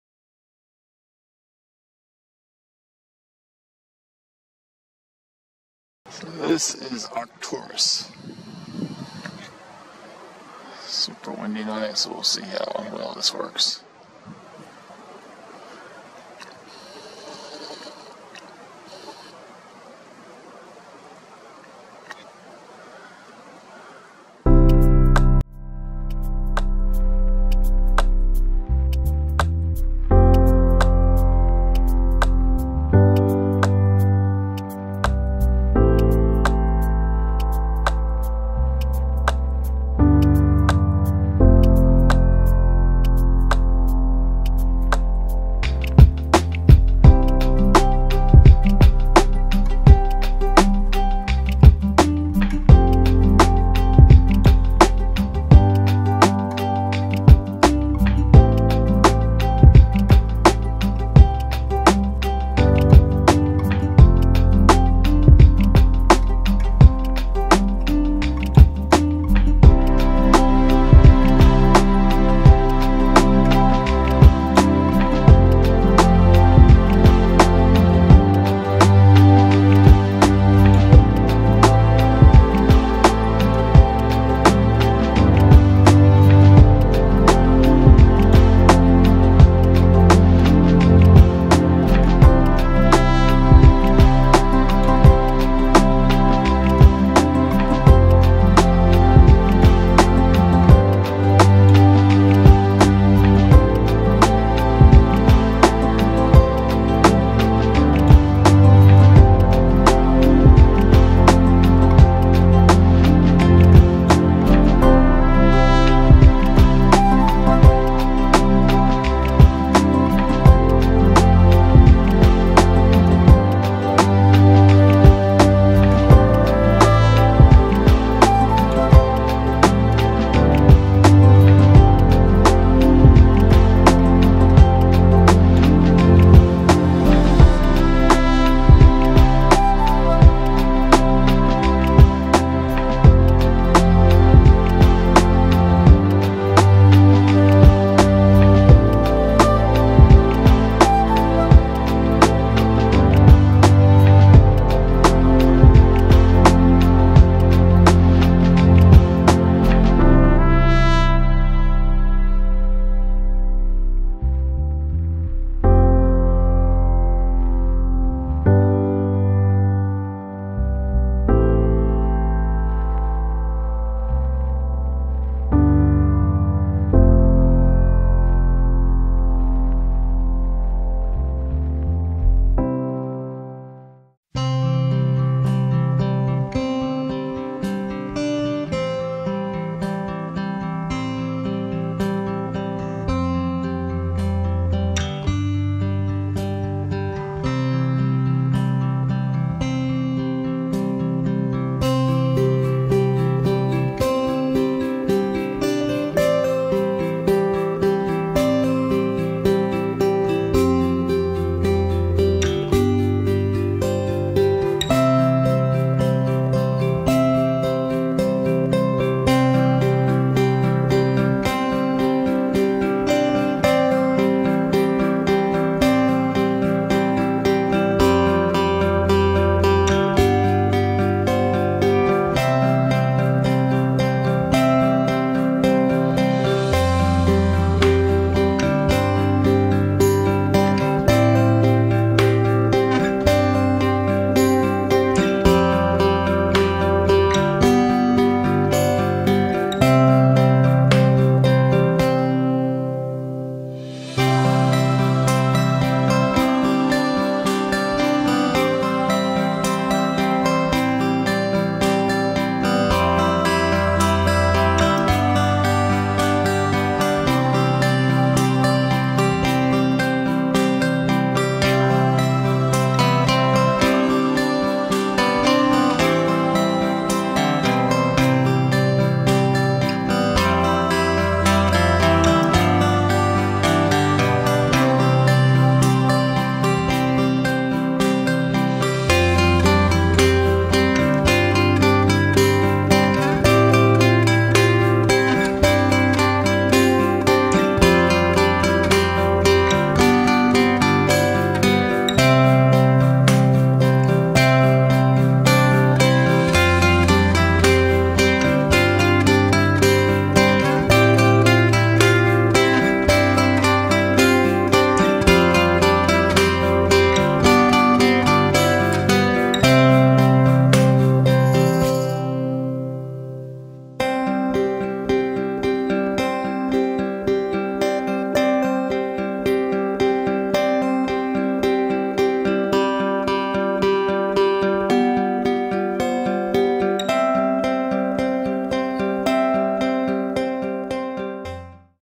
Arcturus: Firmament Luminary or Burning Ball of Gas?
A short video looking at the star Arcturus through a Nikon P1000.  Is it really a giant ball of burning hydrogen light years away or something much smaller and closer that was placed in the firmament during Creation?